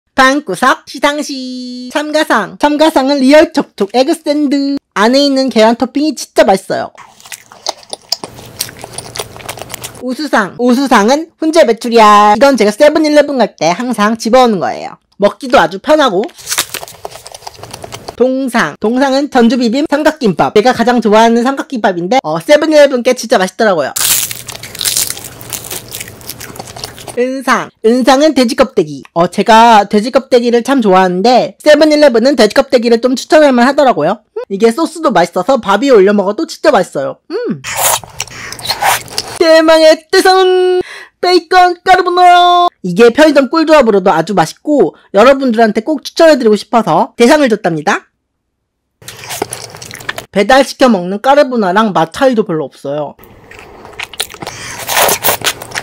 🏆내가 좋아하는 편의점 음식 순위 7eleven편 주관적인 편의점 랭킹+먹방! 제리얌의 방구석 시상식🥉🥈🥇 Convenience store korea
편의점 순위 랭킹 간편식 제리얌 맘대로 정하는 방구석 시상식 7eleven편! 제리얌만의 편의점 꿀팁 음식 순위를 소개합니다! 여러분들은 7eleven편의점에서 어떤걸 가장 자주 먹나요? (광고를 포함하고 있지 않습니다)

제 먹방을 재밌게 보셨다면 [구독]과 [좋아요]를 눌러주세요!